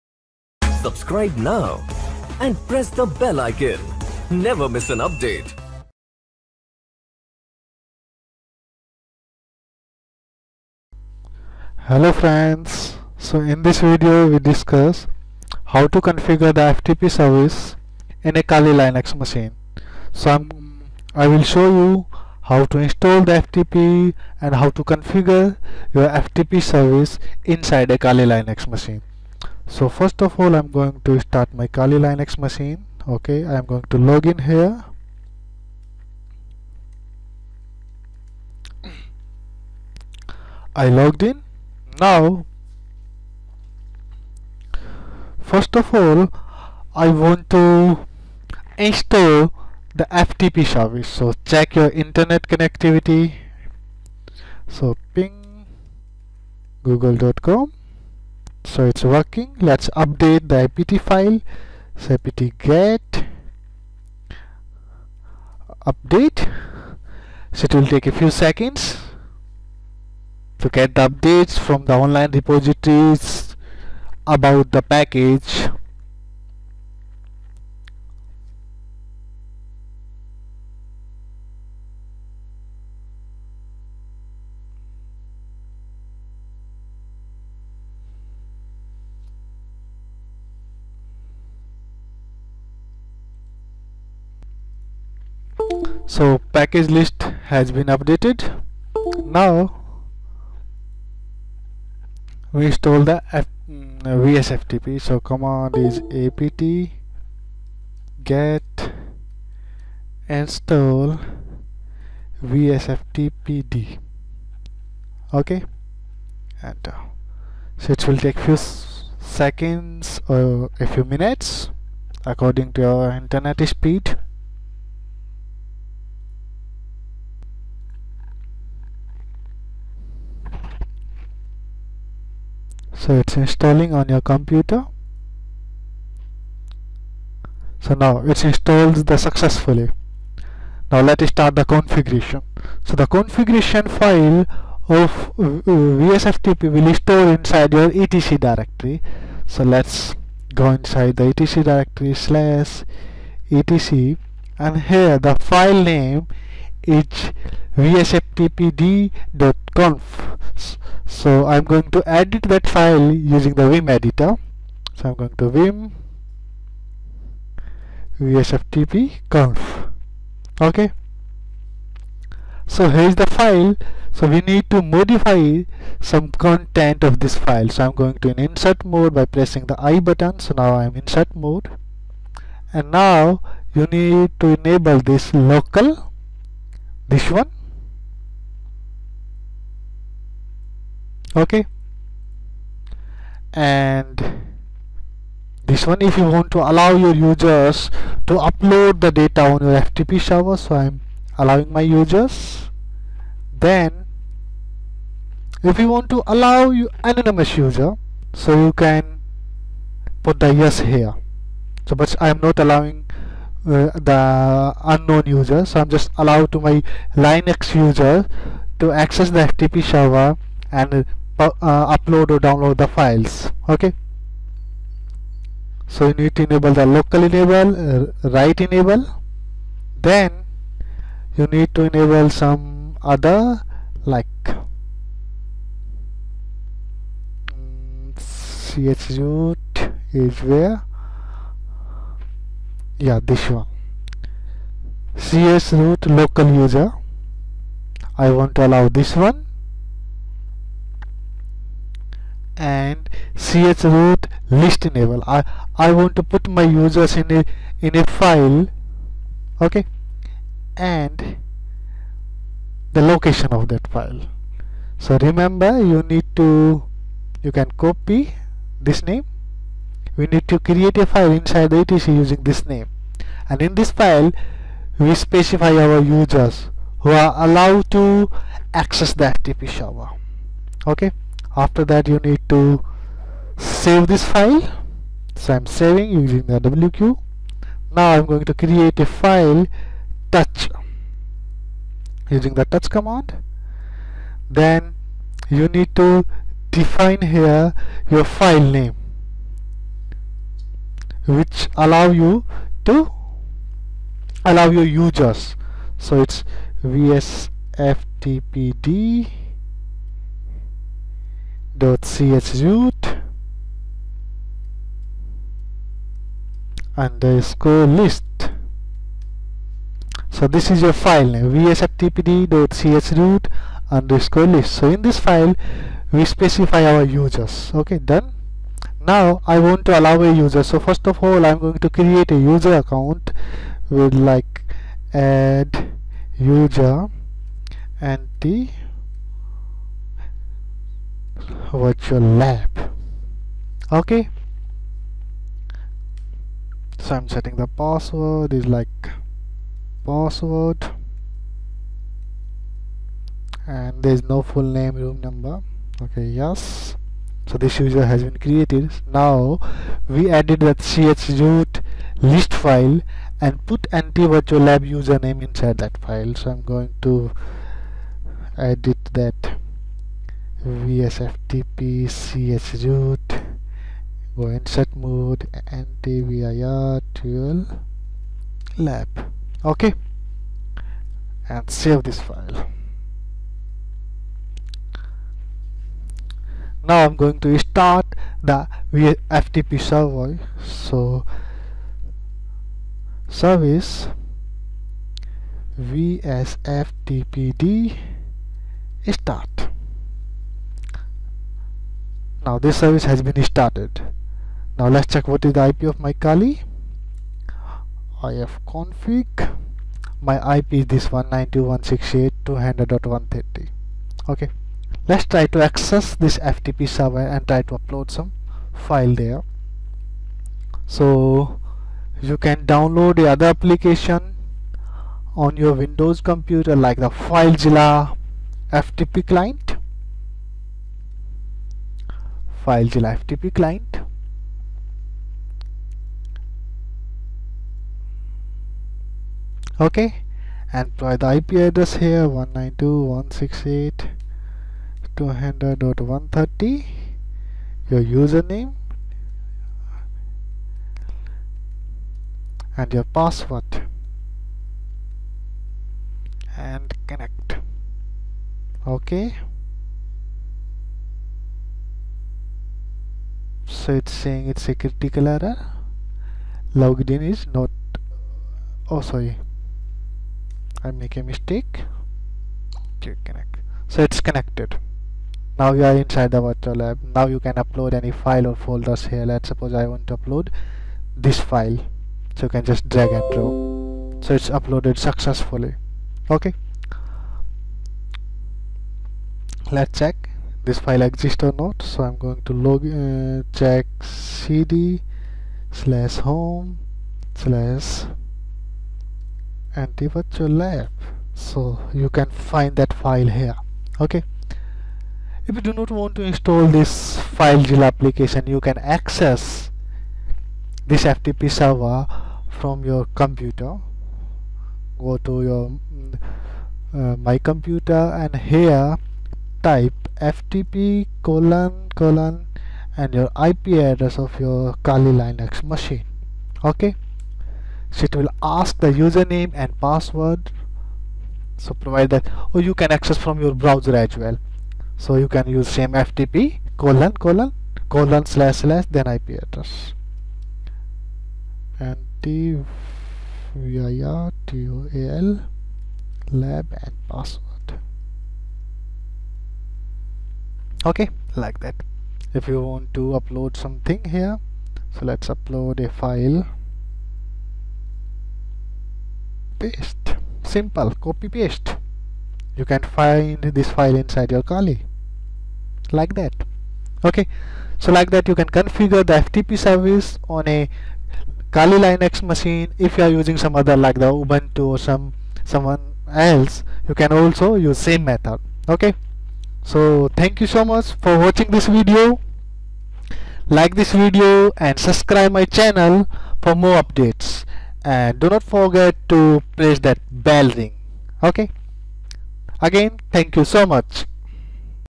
How to configure FTP server on Kali Linux|vsftpd
Learn to configure FTP service inside kali Linux Machine in just few simple steps. Mainly focused on ftp service package inside Kali Linux and configuration file of ftp service. Also learn how to manually allow specific user to access your ftp services